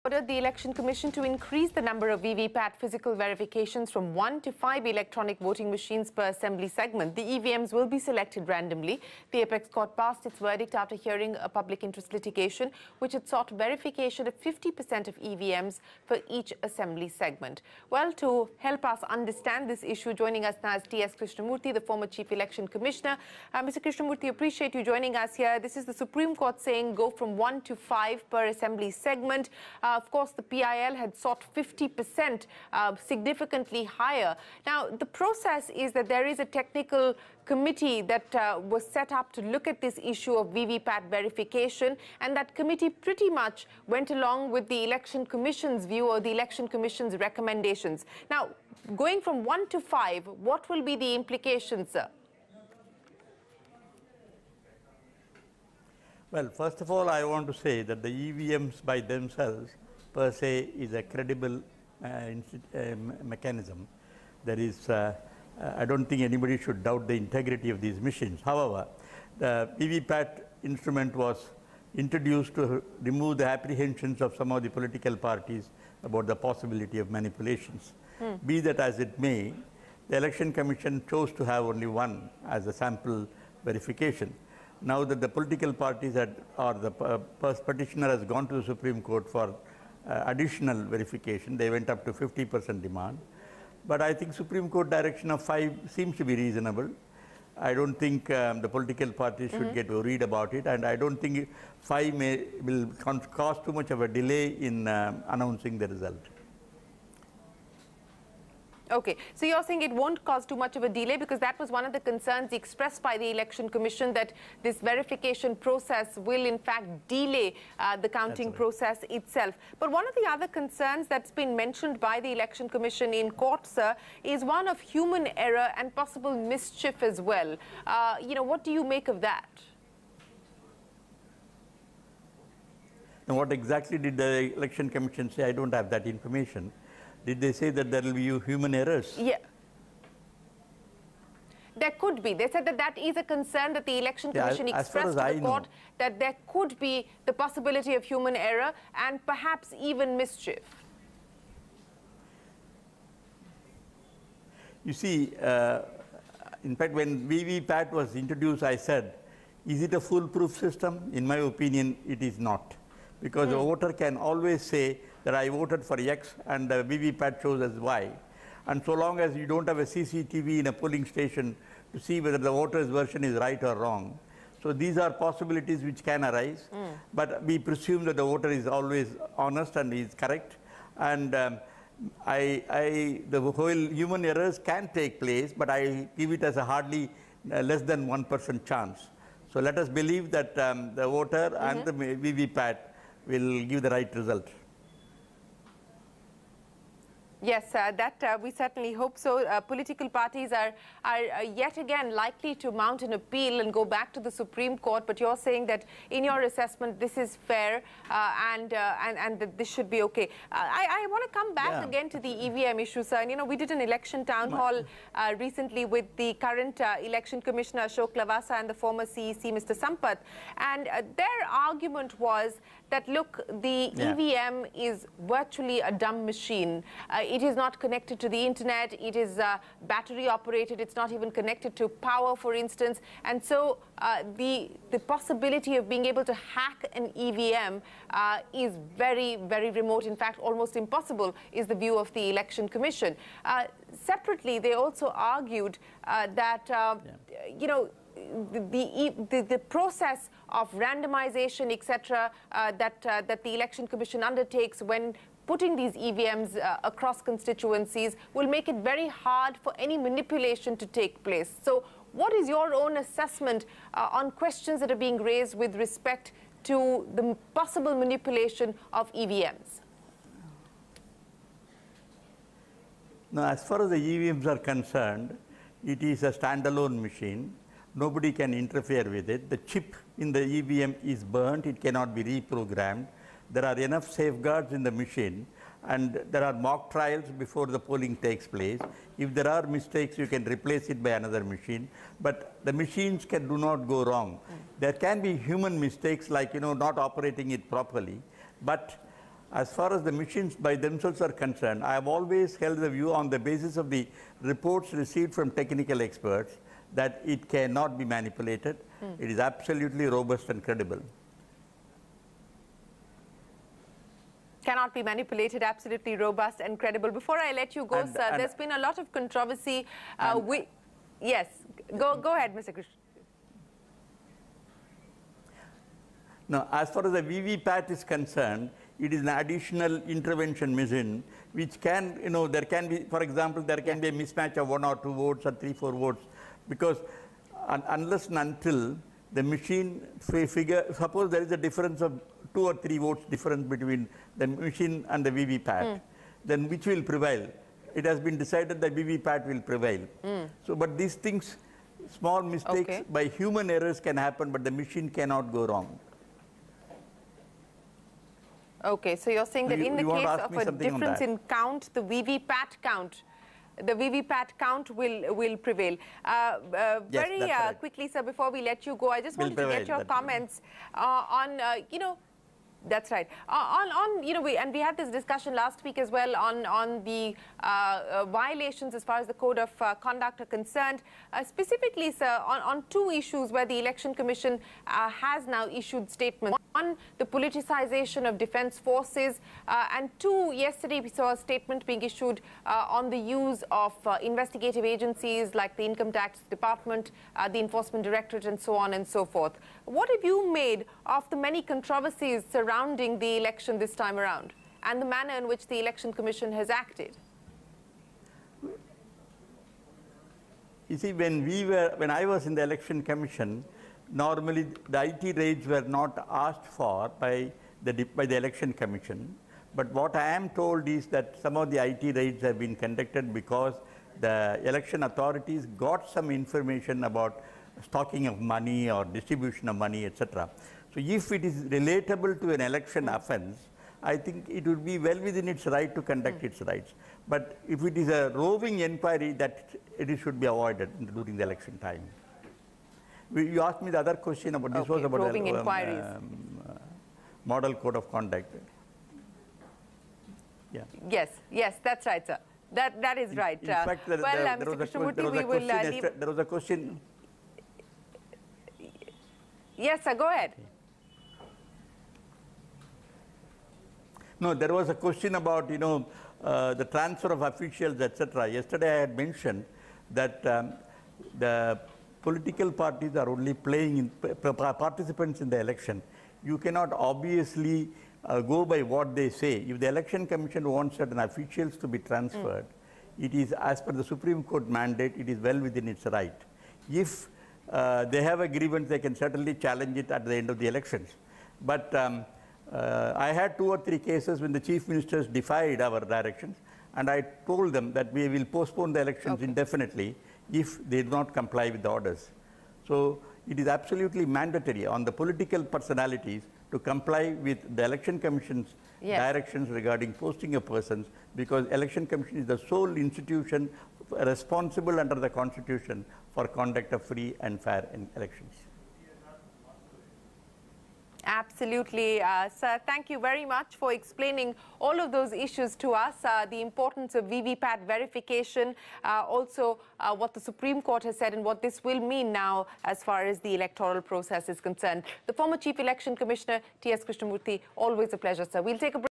Verify 5 EVMs/Assembly Segment: SC On VVPATs
SC order on VVPATs: Increase verification from 1 EVM to 5 EVMs per assembly segment. Shereen Bhan talks to TS Krishnamurthy, Former CEC to get his views. Elections2019